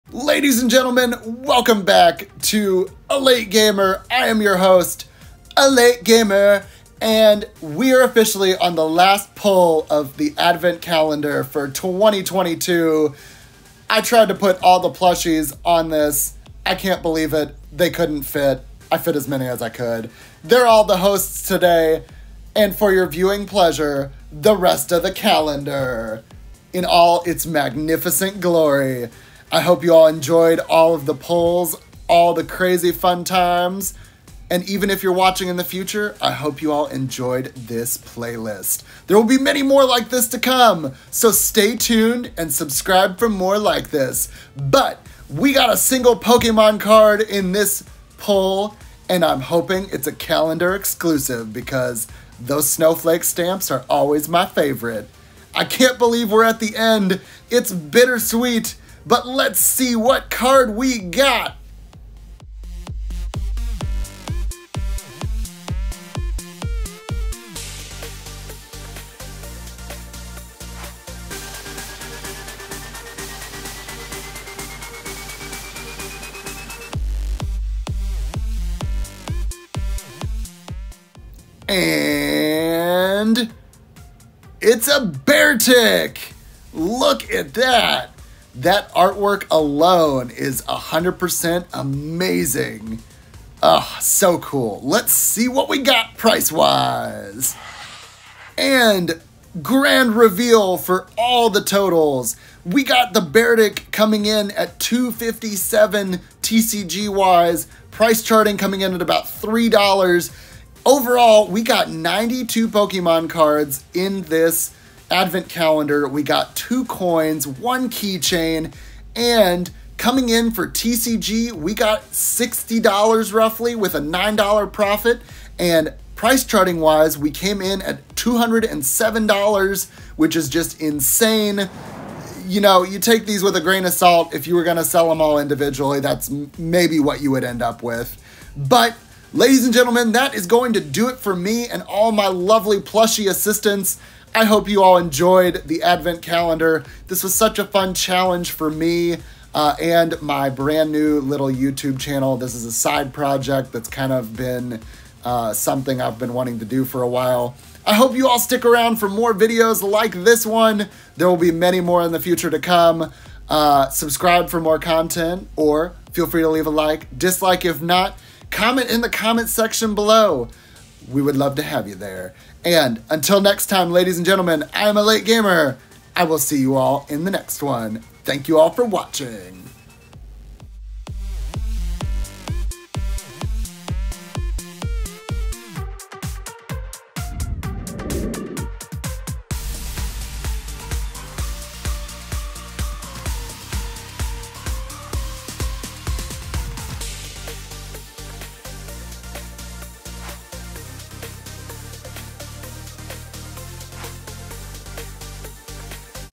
Pokémon Holiday Calendar 2022 - Pack 25 of 25 (Holiday 2022 Advent Calendar Exclusive)
Welcome Everyone! Happy Holidays we are officially at the end of this historic series for my channel! I can't believe it! Due to time constraints I went ahead and shot these videos ahead of time and am dropping them all on Christmas Eve of 2022! Episodes 19-25. I hope everyone has had an amazing time! I am also going to do a 26th episode for this series going over all the cards again but in one video! That video will probably happen sometime after Christmas or the New Years. But I am so excited to see what 2023 brings. For all my viewers and this channel! I hope you all have a safe Holiday and I will catch you all in the next video!

Chapters
0:00 - Intro
0:30 - Thank you all!
1:24 - Last Card Pull!
1:38 - TCG & Price Charting Info
2:19 - Outro